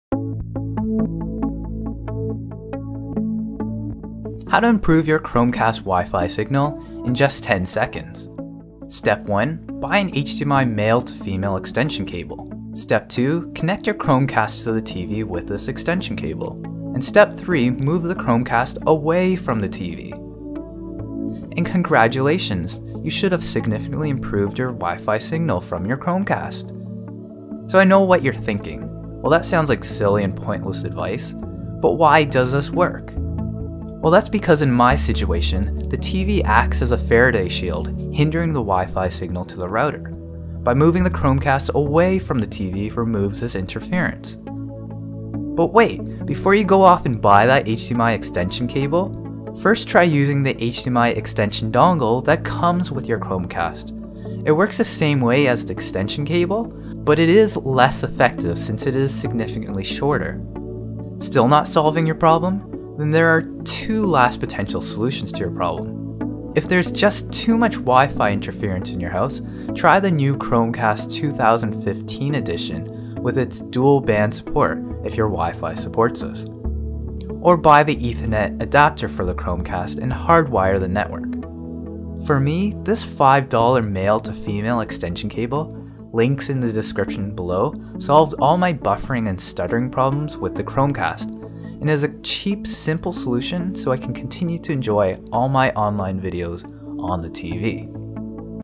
CHROMECAST WiFi Signal Fix (stutter or buffer problems)
How to improve the WiFi signal on the Chromecast original. I had the occasional stutter or buffering issue with my Chromecast (original) and using an HDMI Male to Female extension cable help completely fix that issue for me.

The problem with my setup is that the Chromecast would be hidden behind the TV when connected. And TV acted as a Faraday shield and hindered the wifi signal from the Chromecast. By using an extension cable and moving the device away from the TV significantly improved the throughput and reliability of the signal.

I bought my HDMI male to female extension cable from eBay, but you can find similar cables available from Amazon for less than $5. I went for a 3ft length cable to give me the most flexibility on where I position my chromecast.

As per the alternative suggestions.
The new chromecast 2015 with dual band support can be purchased from the google store.

The chromecast ethernet adapter can be purchased from the google store.